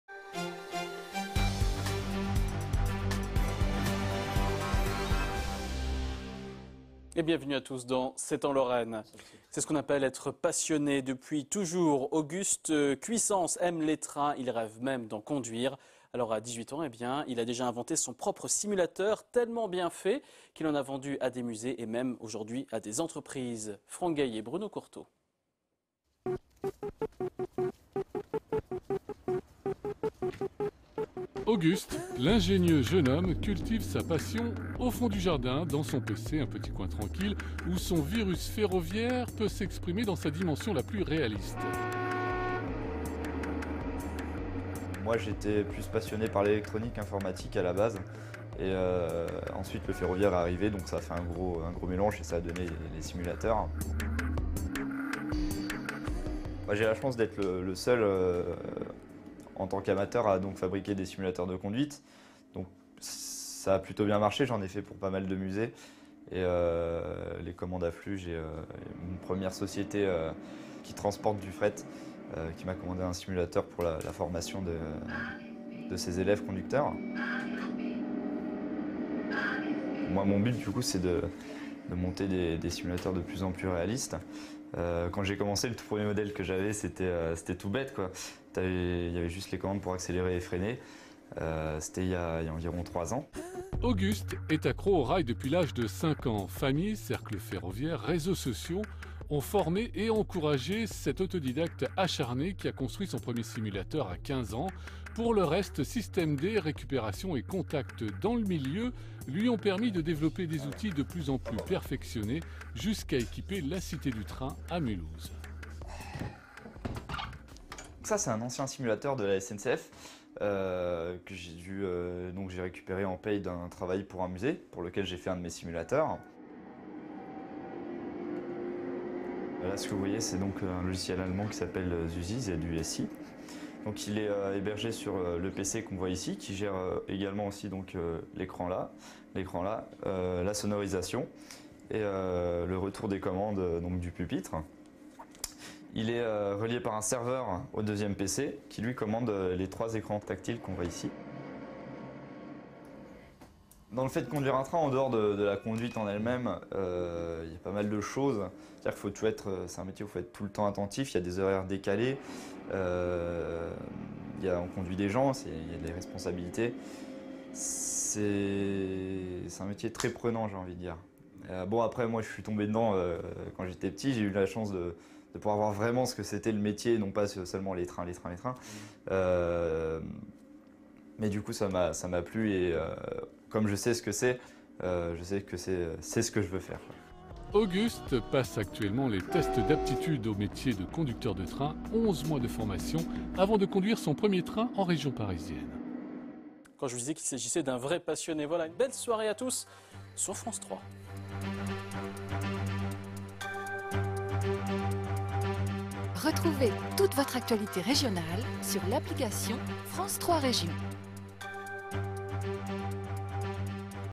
Simulateur de conduite de train SIMCAB - Passage sur FR3 Lorraine
11 ans après le premier reportage, 3 ans après le second, voici de nouveau SIMCAB sur le petit écran !

Remerciements a tous ceux qui ont participés à cette aventure hors du commun.